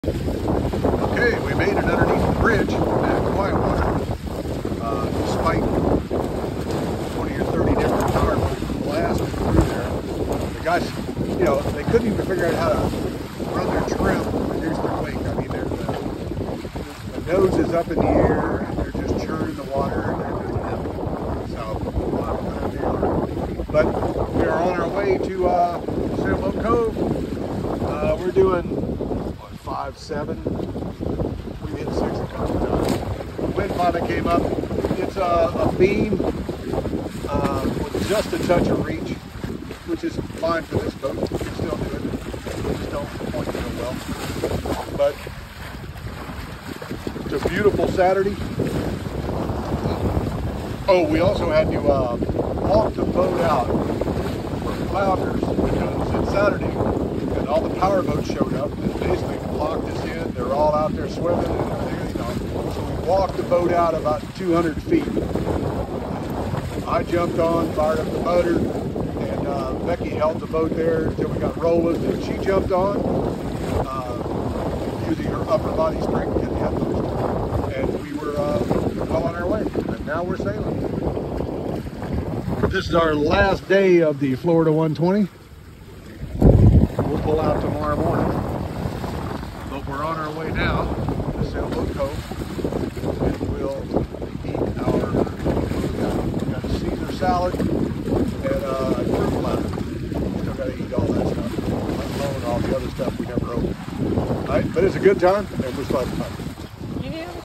Day 3 Saturday afternoon just past Quietwater bridge after getting blasted by power boats
Finally sailing, after walking boat through fleet of power boats/swimmers from Flounder's Beach and motoring under the bridge.  Hit 7 knots regularly near Pensacola Pass enroute west of Sailboat Cove.